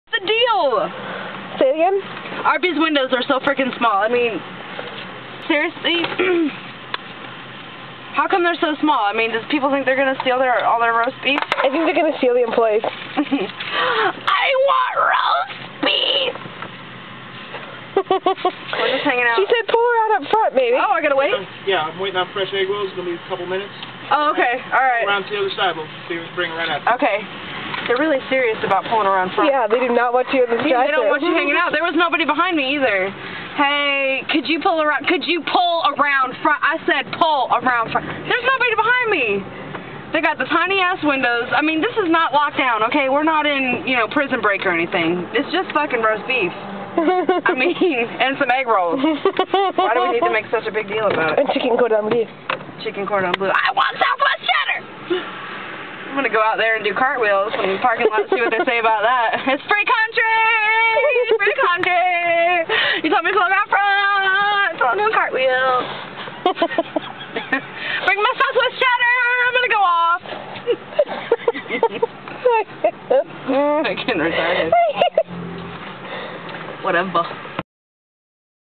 Chris goes off in an Arby's drive through
Why are Arby's windows so friggen small?